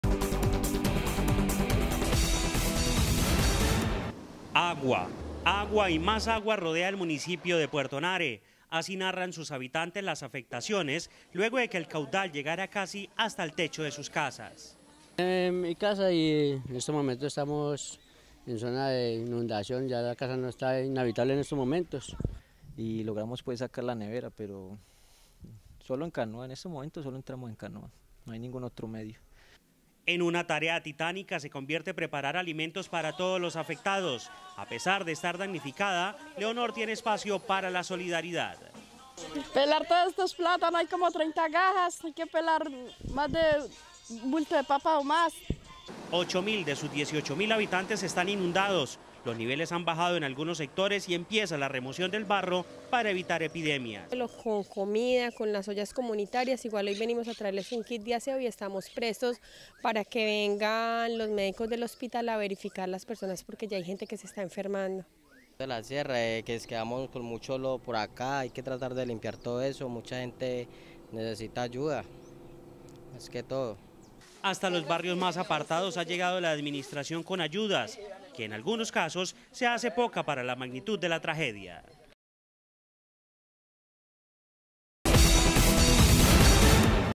Habitantes de Puerto Nare empezaron a recibir ayudas
Seguimos este recorrido por las poblaciones afectadas por las inundaciones. En Puerto Nare, ya los habitantes empezaron a recibir las ayudas correspondientes.

-Teleantioquia Digital-